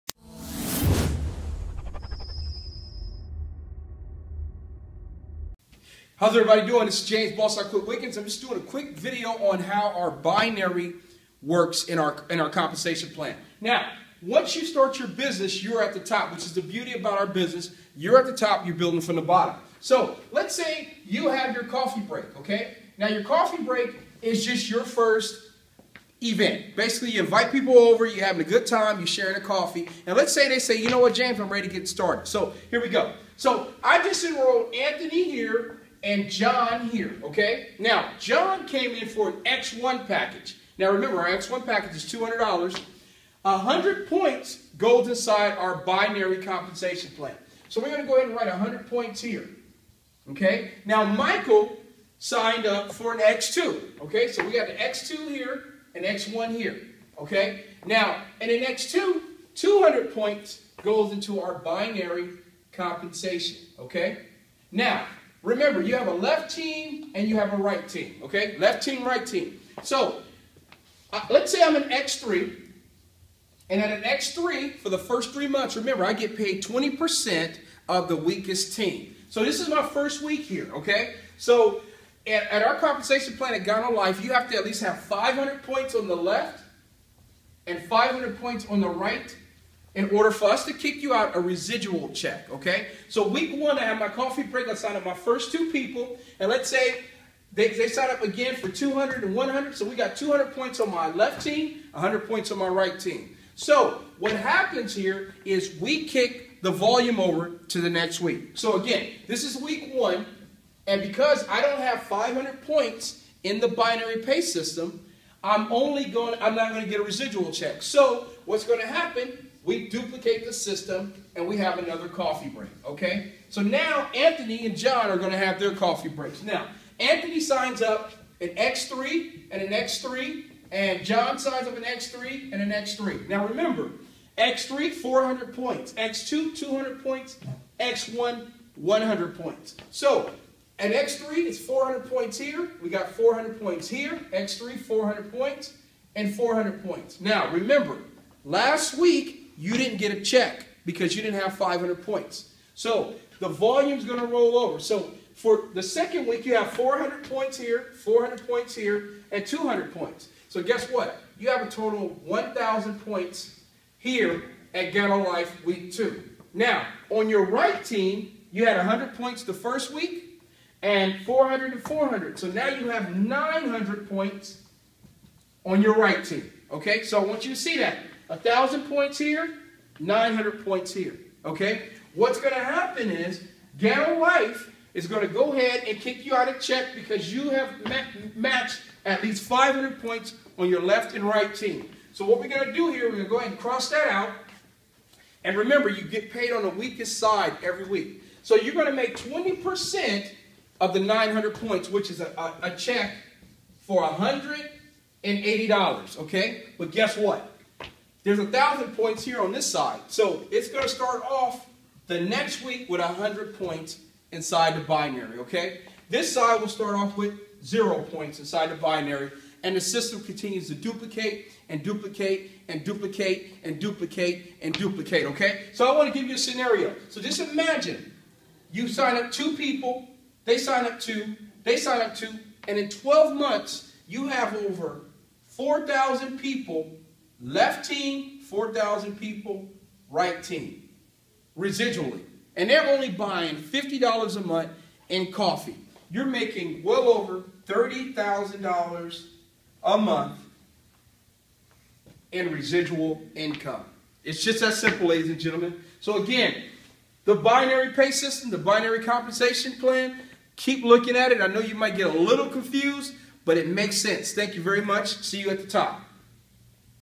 How The Binary Works At GanoLife! With BIQ University Intro
James Wiggins, CEO of BossIQuit, educating on how the binary works at GanoLife!